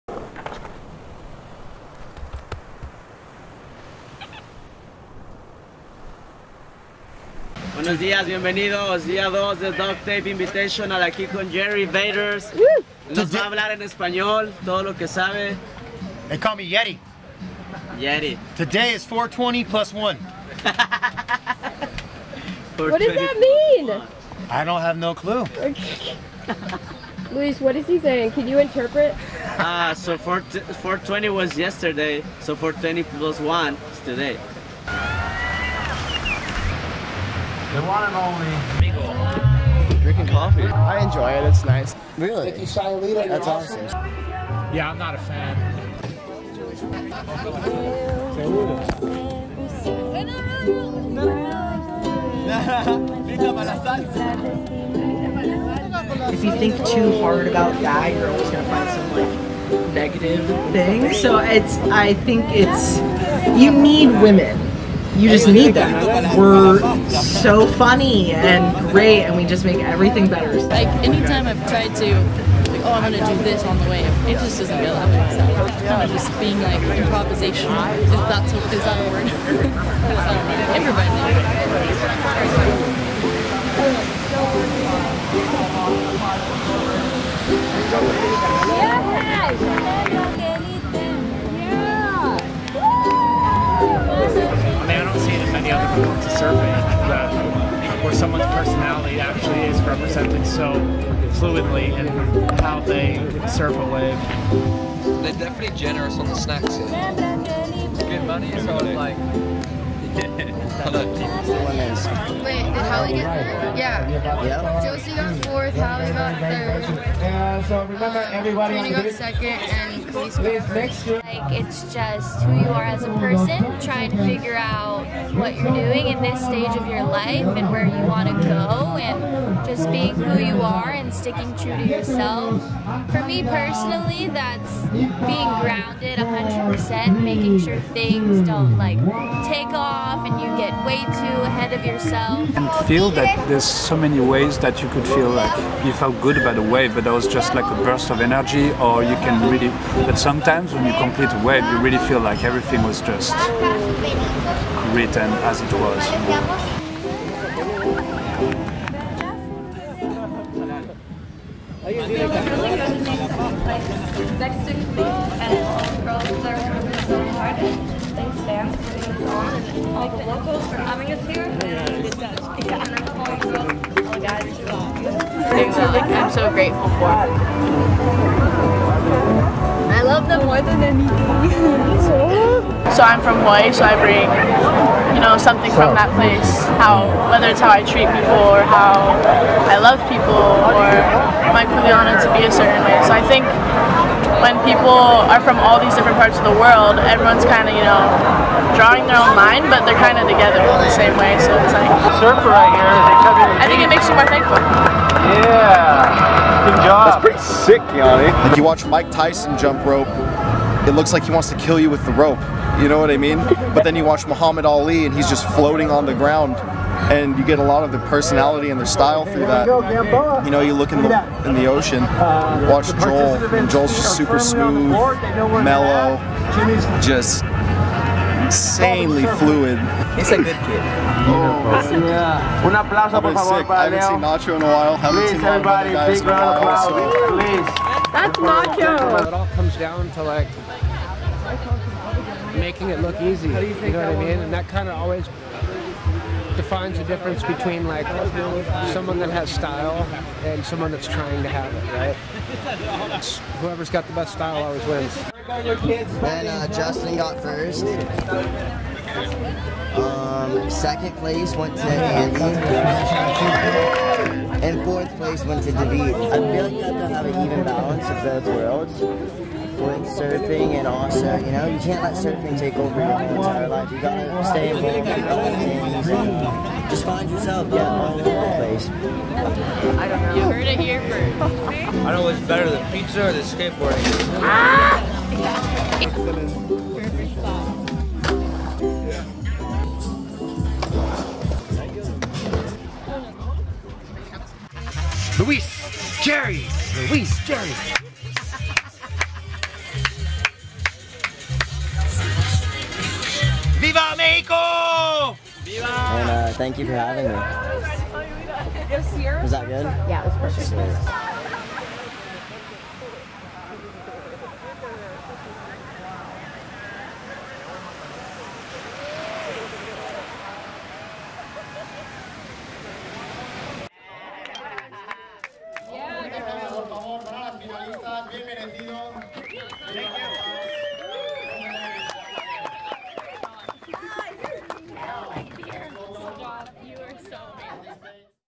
XVan Duct Tape Sayulita 2022 | Beach Scene Compilation
Vans Duct Tape Sayulita 2022 Beach Scene
April 20-23, 2022
Sayulita, Nayarit, Mexico

Filmed and edited by Beth O’Rourke
Camera: Canon C100 Mark 2

Featuring in order of appearance (mostly)

Nathalie Mignot
Luis Medina
Vans Jerry
Grant Noble
Saxon Wilson
Alex Valich
Claire Murphy
Ryan Cannon
Summer Richley
Tosh Tudor
Hallie Rohr
Andrea Sonnenberg
Evan Schell
Andy Nieblas
George Pedrick
Andrew James Peters
Robin Pailler
Summer Richley
Kelis Kaleopa’a
Rosie Jaffurs
Clovis Donizetti
Karina Rozunko
Avalon Gall
Keani Canullo
Chloe Calmon
Kaimana Takayama
Nacho Pignataro
Lola Mignot
Justin Quintal
David Arganda